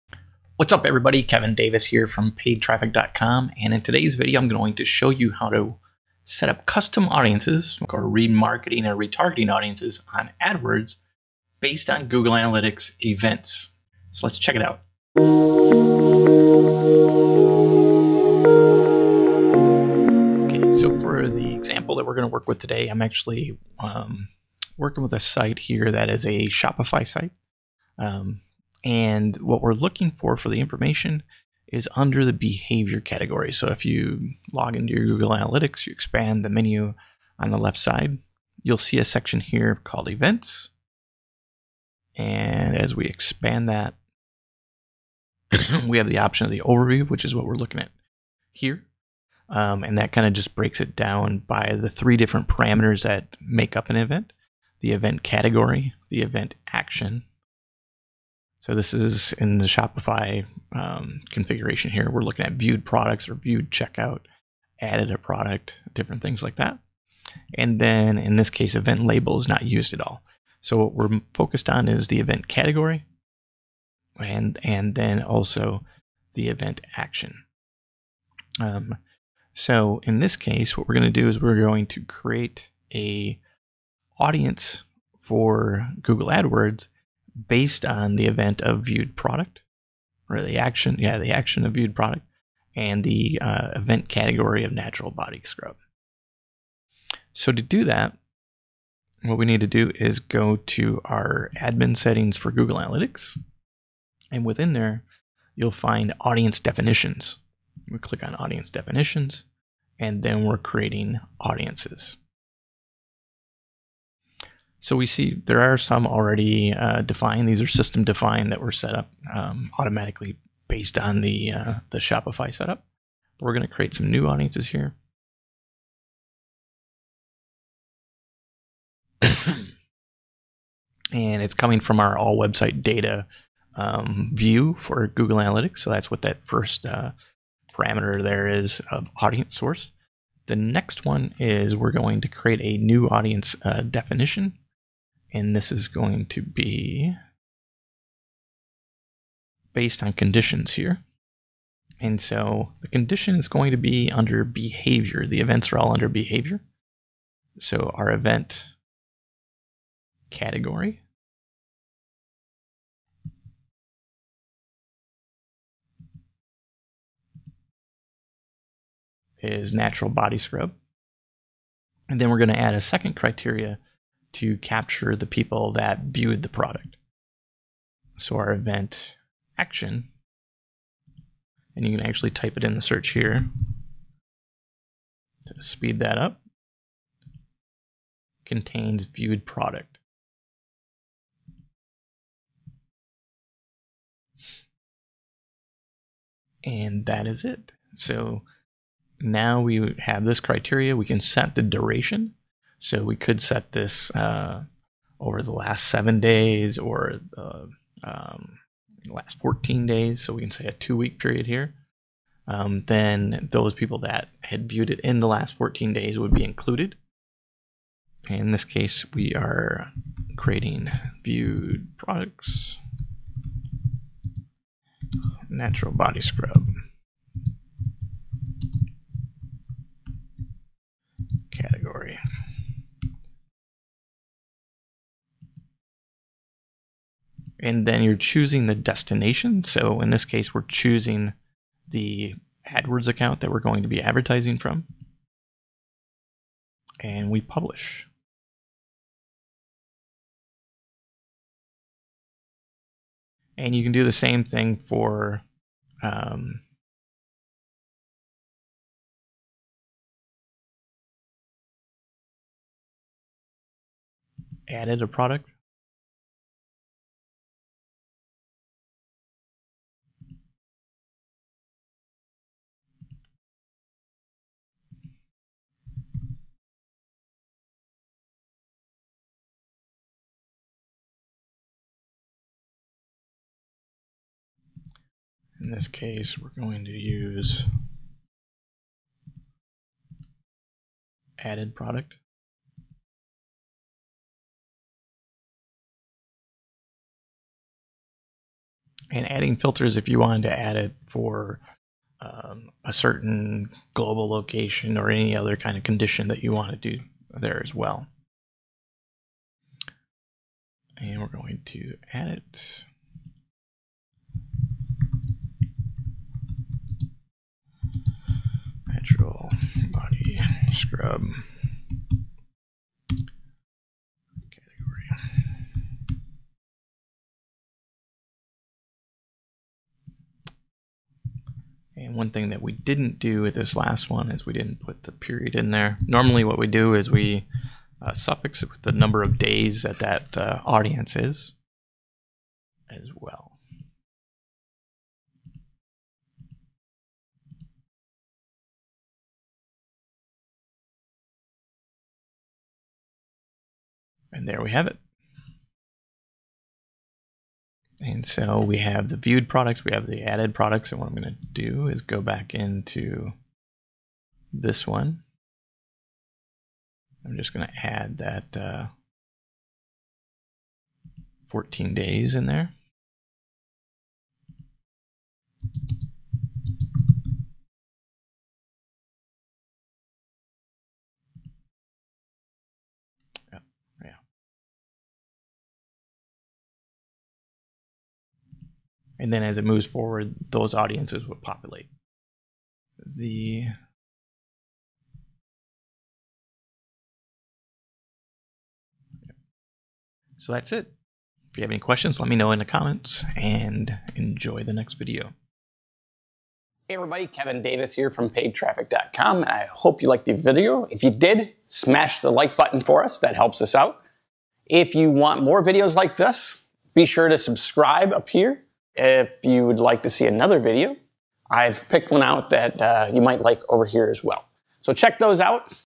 Adwords Remarketing Audiences With Google Analytics Events
In this video, we walk through how to create a remarketing audience in Google Adwords using events captured in Google Analytics.

In the example, I create two retargeting audiences from the traffic to a Shopify Store based on Google Analytics events that occurred when website visitors viewed a product or added the product to their shopping cart.

With Audience Definitions in Google Analytics to can virtually target any action or attribute of website visitors captured by GA in Google Adwords.

These same audiences will then show up automatically in the Adwords Audience Manager.